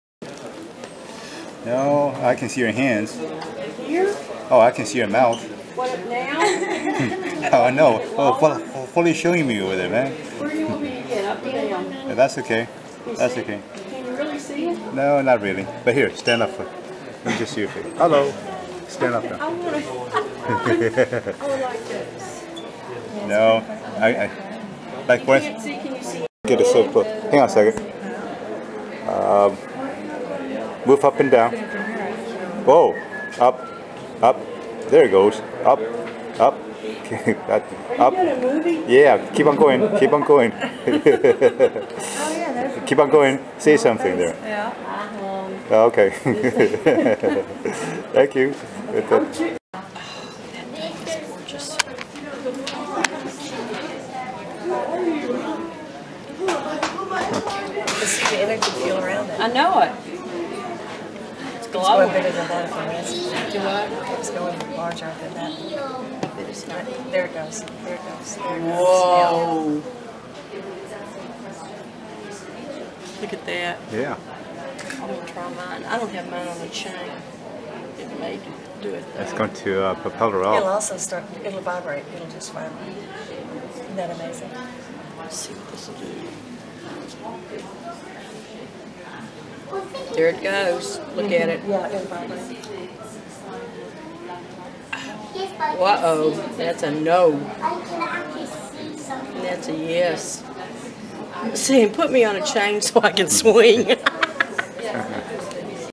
CRYSTALS AT MUSEUM 2014
WASH DC WITH CHERYL AND LEE 2014 MUSEUM OF NATURAL HISTORY